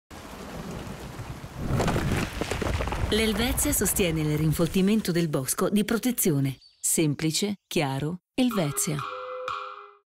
Pioggia torrenziale. Smottamento. Bosco di protezione.
Il 1° novembre 2017 è stata lanciata la nuova campagna del marchio Helvetia in Svizzera. L'elemento centrale della campagna è costituito da triadi scritte che esprimono una chiara promessa: l'Helvetia ti semplifica la vita.

Helvetia Assicurazioni Svizzera: Pioggia torrenziale. Smottamento. Bosco di protezione.